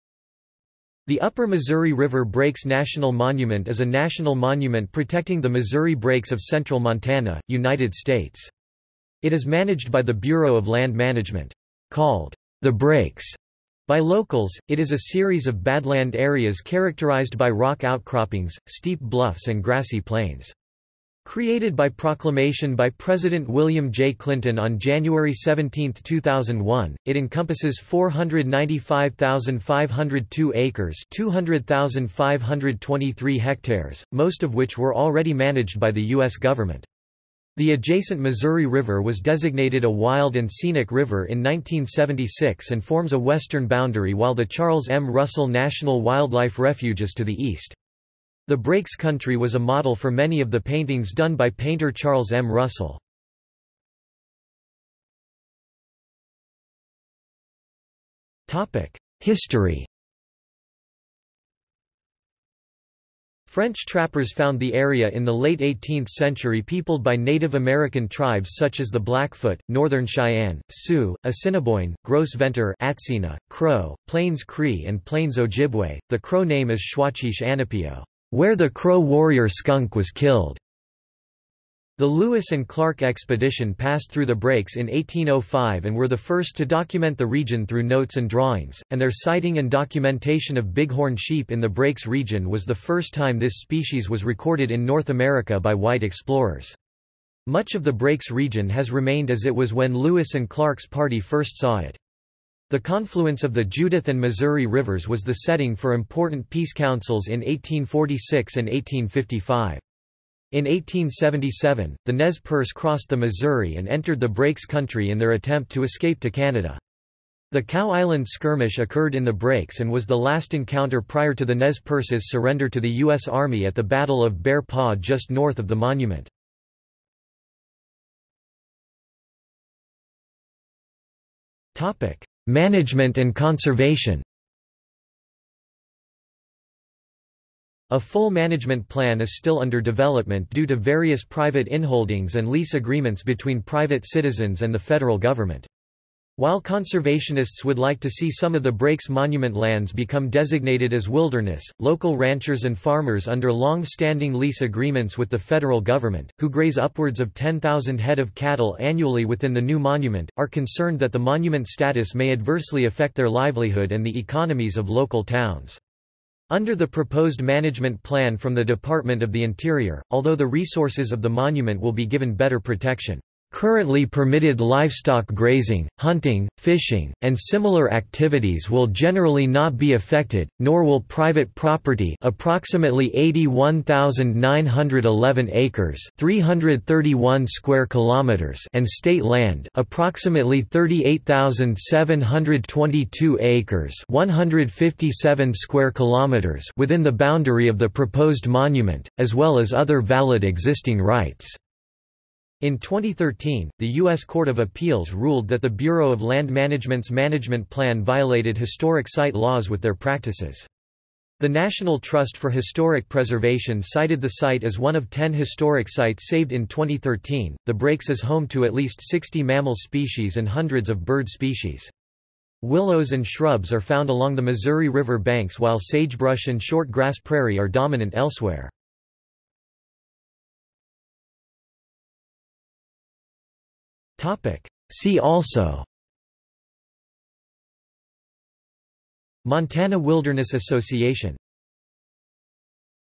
Upper Missouri River Breaks National Monument | Wikipedia audio article
This is an audio version of the Wikipedia Article:
Upper Missouri River Breaks National Monument

00:00:54 1 History
00:02:08 2 Management and conservation
00:03:50 3 See also

- Socrates

SUMMARY
The Upper Missouri River Breaks National Monument is a national monument protecting the Missouri Breaks of central Montana, United States. It is managed by the Bureau of Land Management. Called "The Breaks" by locals, it is a series of badland areas characterized by rock outcroppings, steep bluffs and grassy plains. Created by Proclamation by President William J. Clinton on January 17, 2001, it encompasses 495,502 acres (200,523 ha), most of which were already managed by the U.S. government. The adjacent Missouri River was designated a Wild and Scenic River in 1976 and forms a western boundary while the Charles M. Russell National Wildlife Refuge is to the east. The Breaks country was a model for many of the paintings done by painter Charles M. Russell.